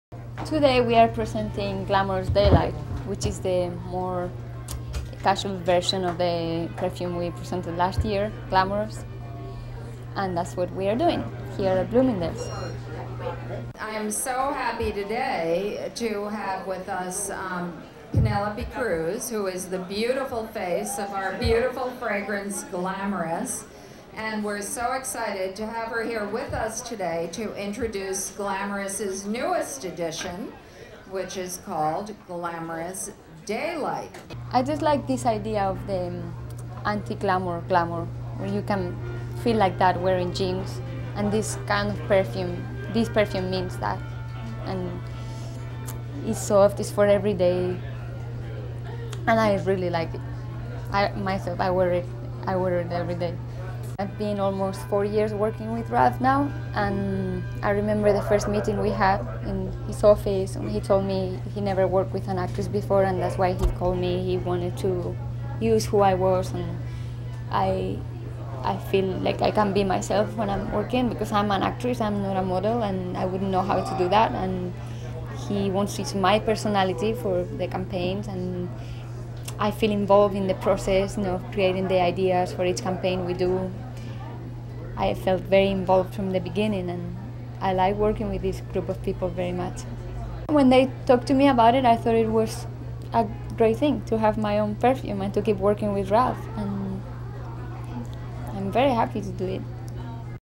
Latest Celebrity Gossip - Penelope Cruz's Glamorous Daylight Perfume Launch with Ralph Lauren
The latest in celebrity gossip Movie star Penelope Cruz talks about her new perfume with Ralph Lauren called "Glamorous Daylight" which she says is a lighter version of the perfume they released last year.  For more celebrity news, gossip, and red carpet interviews check out more from SnapSnap.